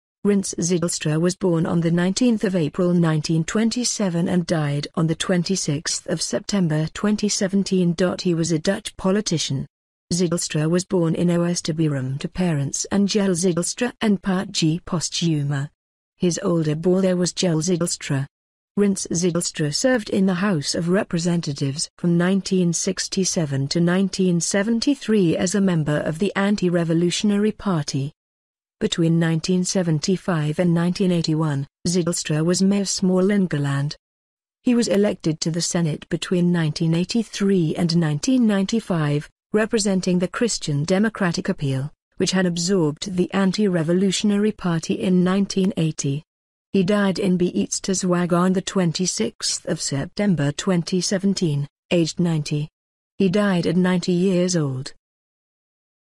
Dutch politician Rinse Zijlstra passed away at 90
Rinse Zijlstra was born on 19 April 1927 and died on 26 September 2017.He was a Dutch politician. Zijlstra was born in Oosterbierum to parents Ane Jelle Zijlstra and Pietje Postuma. His older borther was Jelle Zijlstra. Rinse Zijlstra served in the House of Representatives from 1967 to 1973 as a member of the Anti-Revolutionary Party. Between 1975 and 1981, Zijlstra was mayor of Smallingerland. He was elected to the Senate between 1983 and 1995, representing the Christian Democratic Appeal, which had absorbed the Anti-Revolutionary Party in 1980. He died in Beetsterzwaag on 26 September 2017, aged 90. He died at 90 years old.Get the latest news on famous dead actor, musician, football players, entertainers, celebrities, singer, athletes and much more. goo.gl/KPRcpz     Website= goo.gl/KstE9J